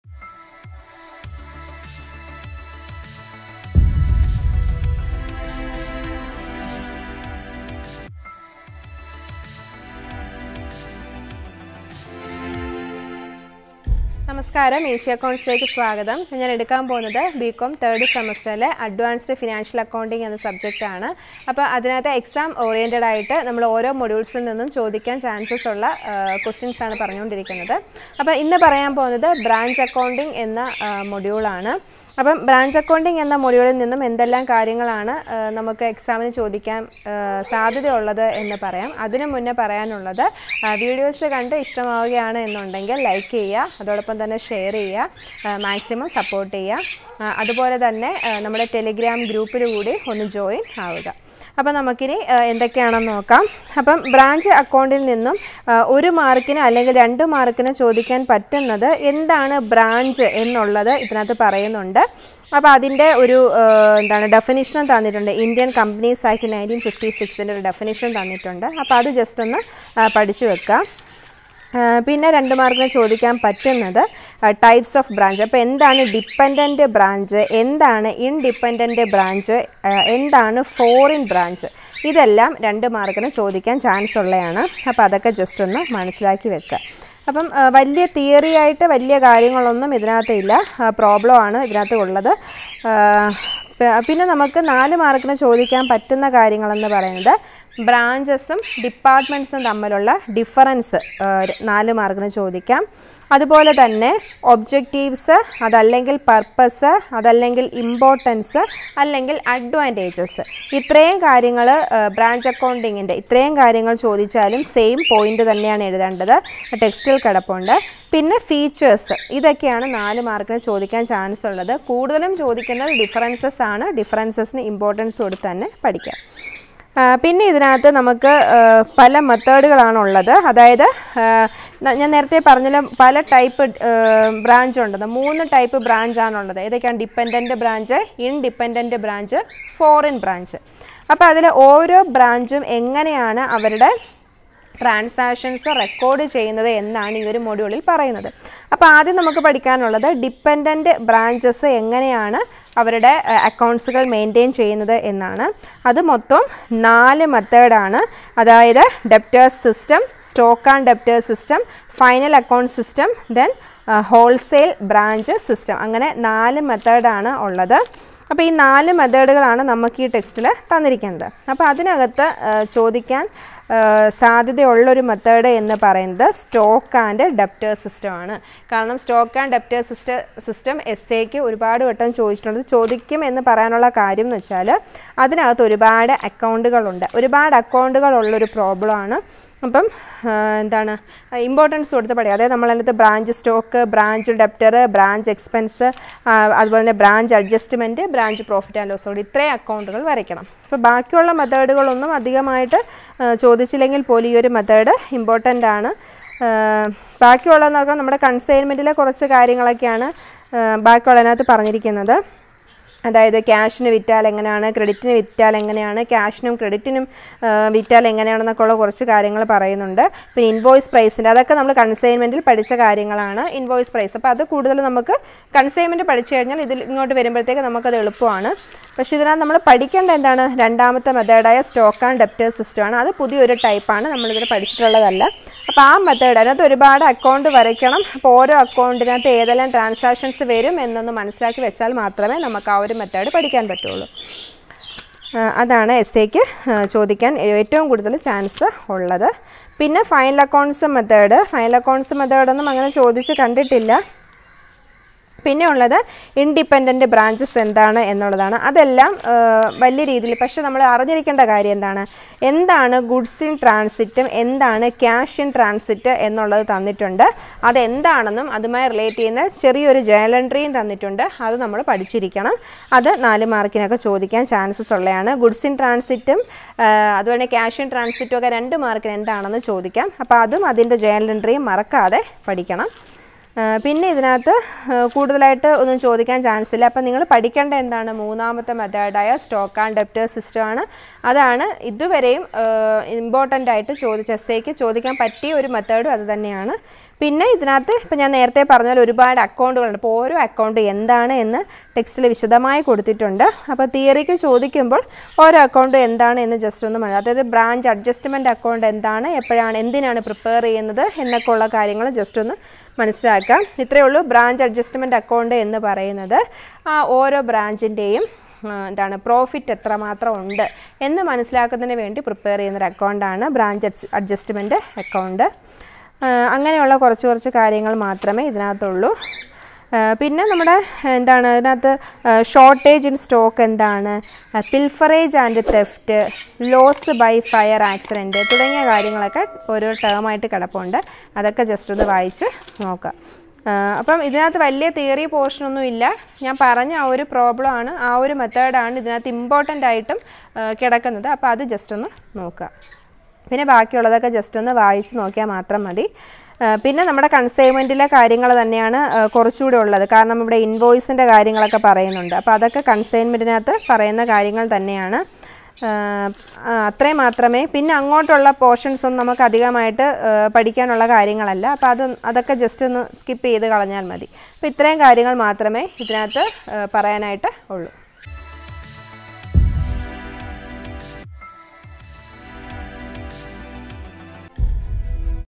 Exam Important Topics | Degree 3rd Sem | Advanced Financial Accounting | Module -4 | Easy Accounts
EXAM REVIEW | Degree 3rd Sem | Advanced Financial Accounting| Module -4 | Branch Accounting | Exam Important Topics | | Easy Accounts |

Class          : Degree 3rd Semester
Subject       : Advanced Financial Accounting |
Chapter      : Module - 4  | Branch Accounting   |
Topic          :  Exam Important Topics |

By,
Easy Accounts Team.